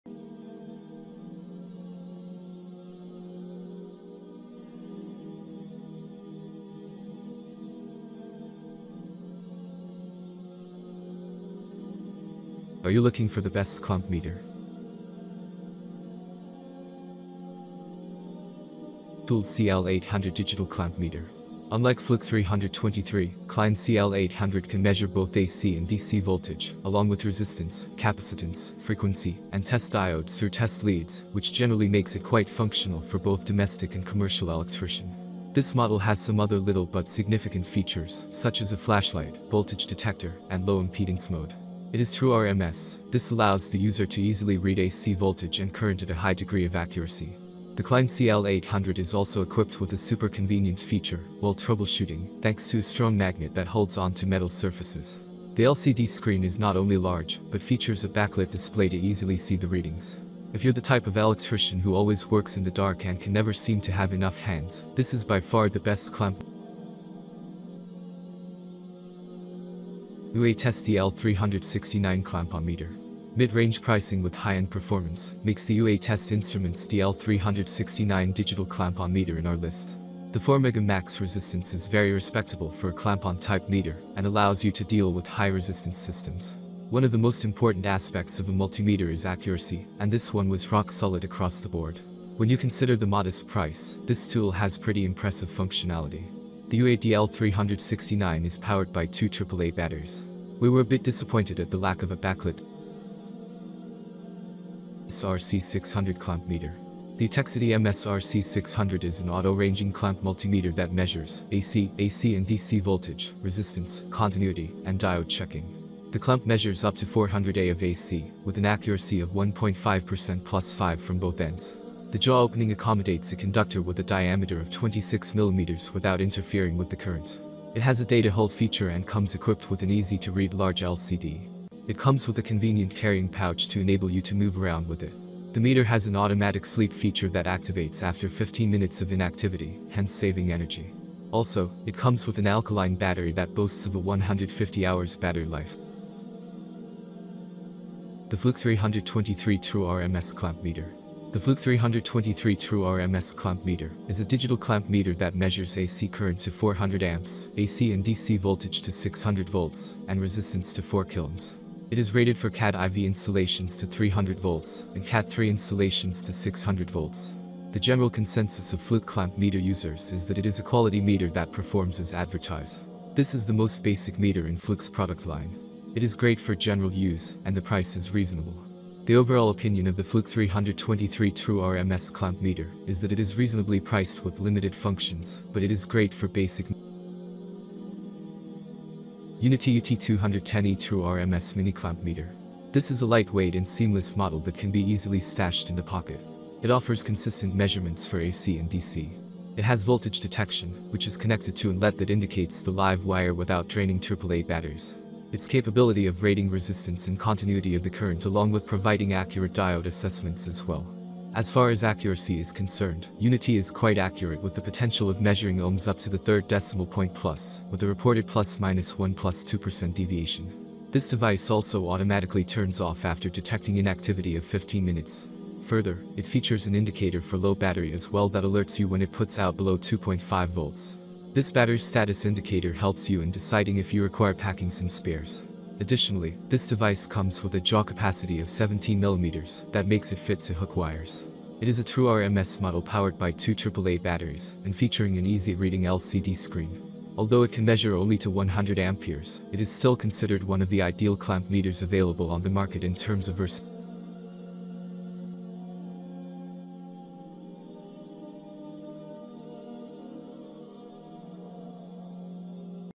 Top 5 Best Digital Clamp Meter 2022 | Best Clamp Meter Review
5. Klein Tools CL900 Digital Clamp Meter, Autoranging TRMS, AC/DC Voltage/Current, 2000A, LoZ , Continuity, Frequency, NCVT, Temp, More, 1000V

4. UEi DL369 Digital Clamp Meter HVAC Auto-Ranging 4000 Counts Volt Meter AC Amp Meter Measures AC Current AC/DC Voltage & Microamps Capacitance Resistance Diodes NCV Continuity Tester Live Wire Test

3. Etekcity Digital Clamp Meter Multimeter AC Current and AC/DC Voltage Tester with Amp, Volt, Ohm, Continuity, Diode and Resistance Test, Auto-Ranging, Red, MSR-C600

2. Fluke 376FC AC/DC Clamp Meter with iFlex For Industrial/Commercial Electricians, VFD Low Pass Filter For Accurate Measurements, Inrush Measurements, Bluetooth Connectivity For Remote Measurements

1. IDEAL INDUSTRIES INC. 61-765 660 Amp TightSight Clamp Meter AC/DC with TRMS, True RMS Current and Voltage, CATIII for 1000v, CATIV for 600v